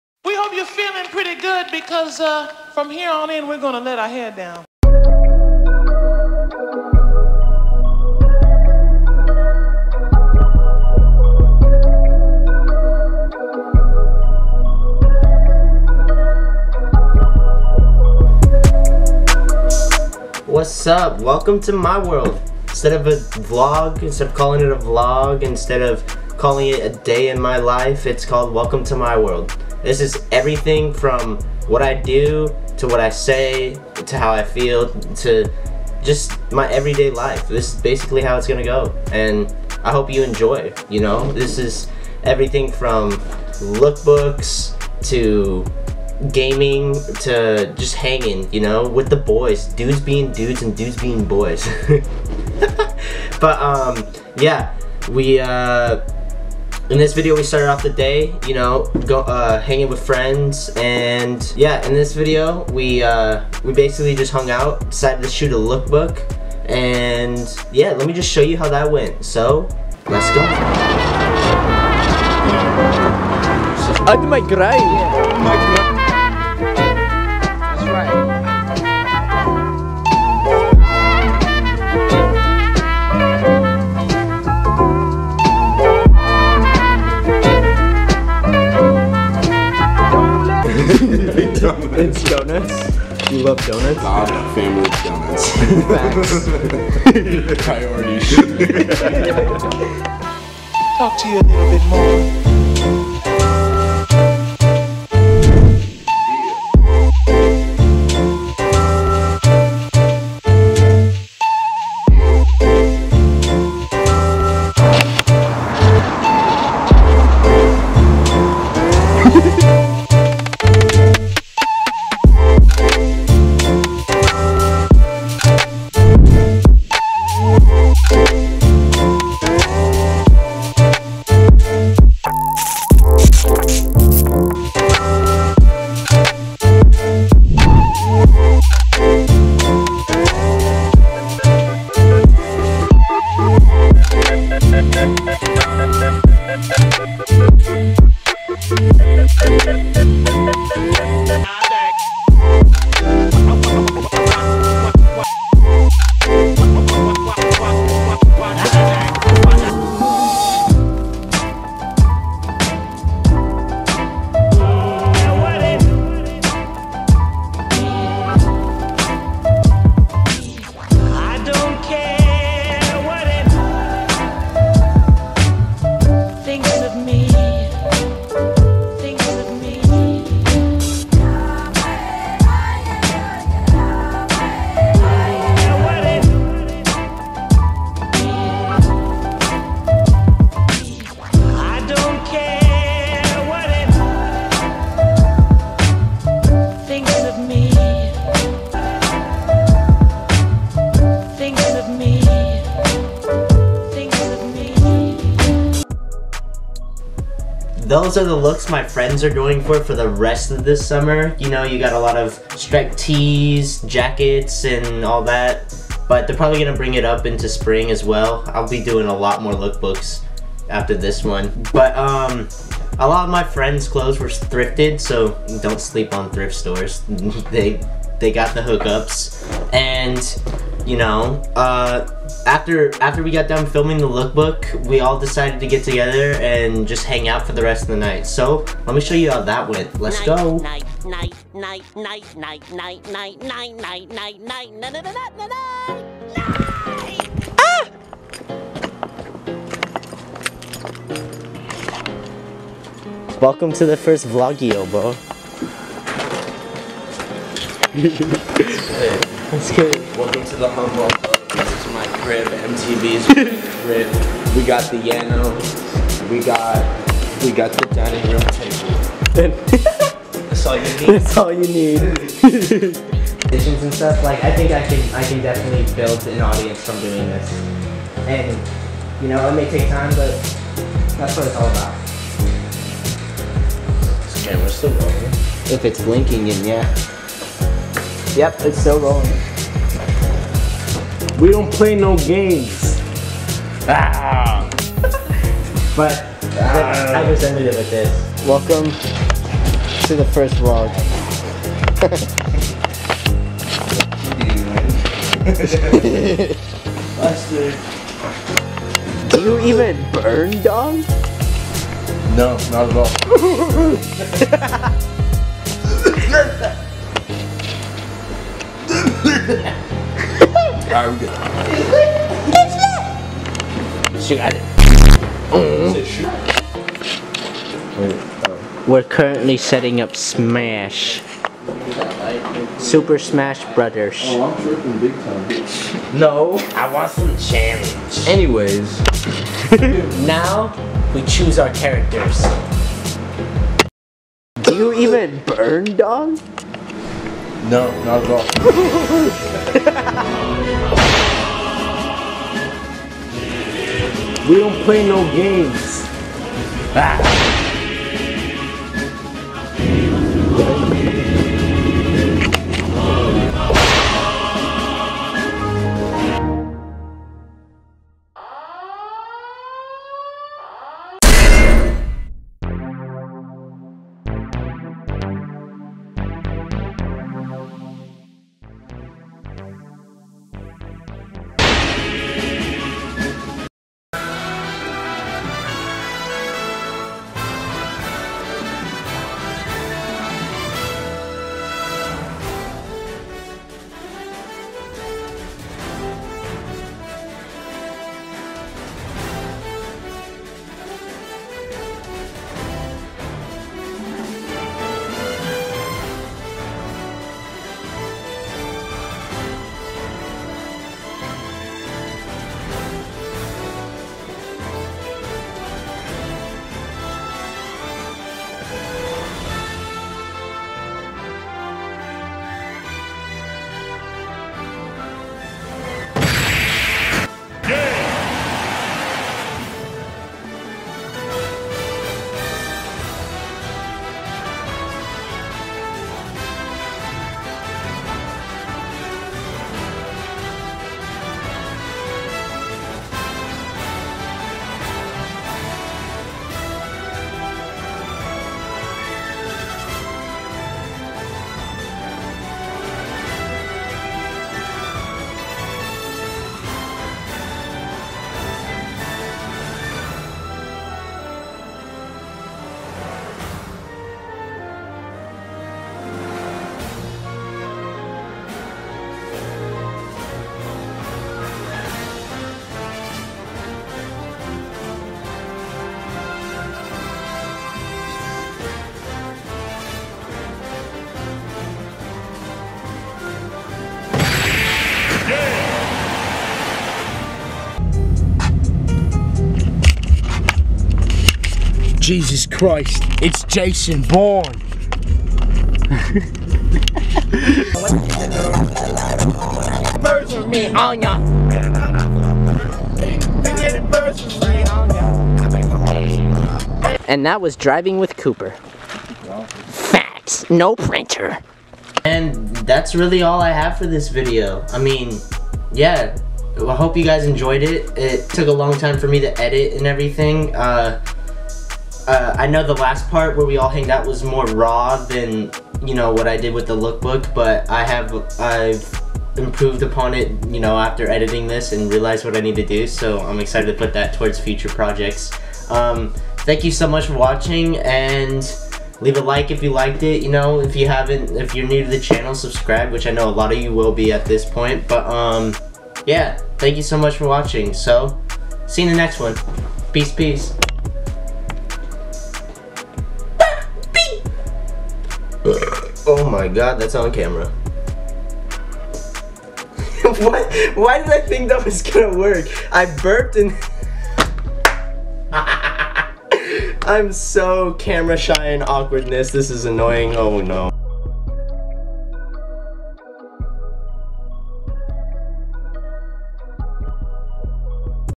WELCOME TO MY WORLD (EP.1)
This is a little taste of my everyday life. from look-books to hanging out with friends.

_______              _________                                                   _________               _______                                       _________                                  STORE COMING SOON                  __________
   ___               _                                    “ MINI “                                                ___  _

( CHECK OUT HIS MUSIC)

                                /    \ /     \    USIC  IN VIDEO   I
                              /                \                               I